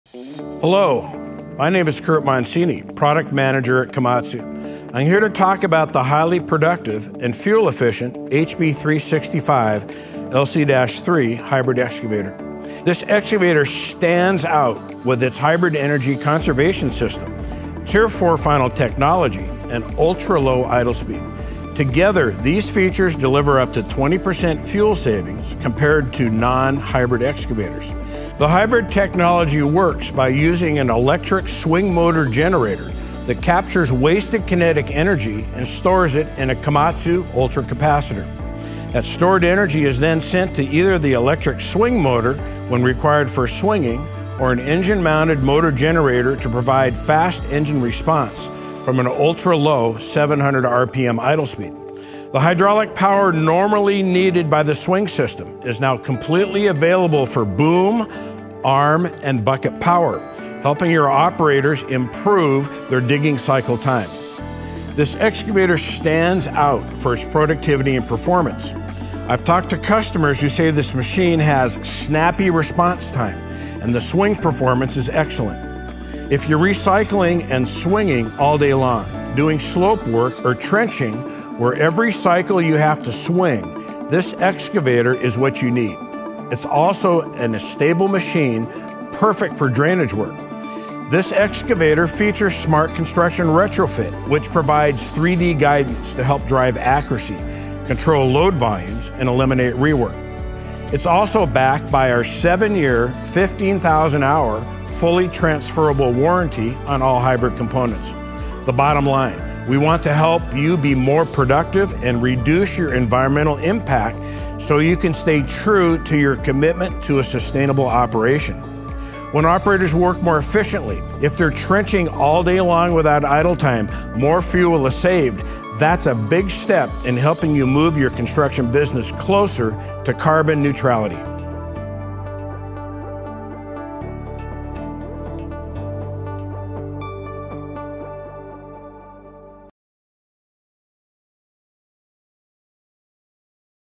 Drive productivity, job-site performance and decrease fuel consumption with the Komatsu HB365LC-3
If your construction business needs higher production with less fuel costs on your journey to sustainability, check out Komatsu's HB365LC-3 hybrid excavator. The HB365LC-3 features a powerful, responsive swing system and a Tier 4 Final engine that provides up to 20% fuel savings compared to non-hybrid excavators. This excavator also features Smart Construction Retrofit, which provides operators in the field (and managers in the office) access to 3D design and topography data that helps drive accuracy, control load volumes and eliminate rework.

For a limited time through Komatsu Financial, get 0% for 36 months or 1.99% financing for 48 months on the HB365LC-3. Contact your local dealer for details.